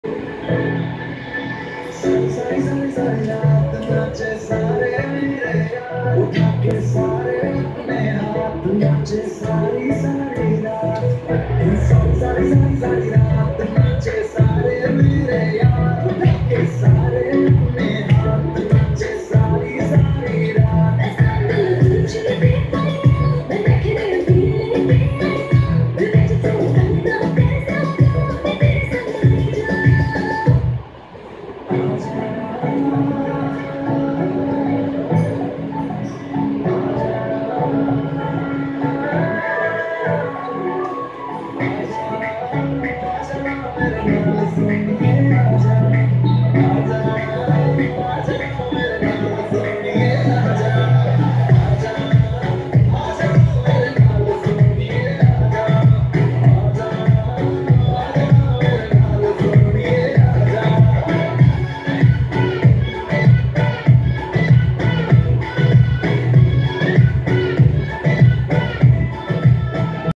Sakshi P | Western Solo | Zest 2021
Zest - IIIT Hyderabad's inter-college dance competition.